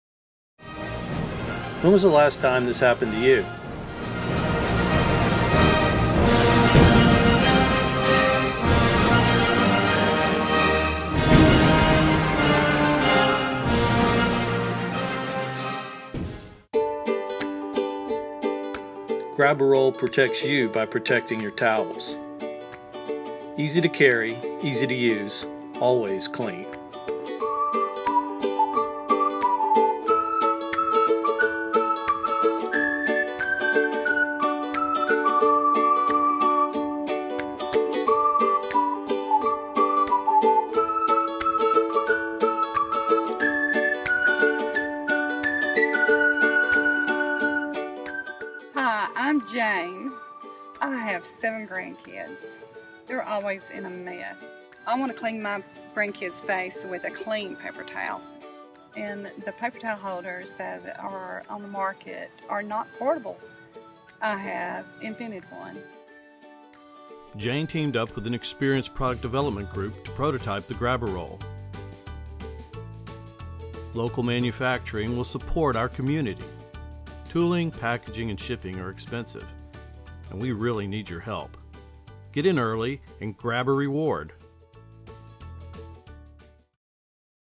The World's Greatest Paper Towel Saver Holder - Grab.a.Roll to GO
NEW PRODUCT: grab.a.roll portable paper towel dispenser

Grab a Roll the world's first portable paper towel tote keeps your paper towels clean when you're on the go. Grab-a-Roll on the Go.

Remember all those times your paper towels blew over, something spilled on them, or they dropped on the dirty ground? Grab.a.Roll™ is the world's first portable paper towel tote. It is easy to use and always clean, wherever your messy life takes you: camping, bbq, picnics, patio, classroom, cars, boat, hunting, fishing, garage (mechanic towels fit), tailgating, or just about anywhere.
Grab.a.Roll was invented by Jane Lawrence, an exhausted grandmother of seven messy kids, after they had ruined rolls of paper towels by dropping them onto the wet ground. Her desperate search for a portable paper towel dispenser that didn't exist led to her inventing one. The help of a product development team enabled Jane to create a working prototype and a successful new product launch on the crowd-funding website Kickstarter.

It's ok go take the one moment to see our product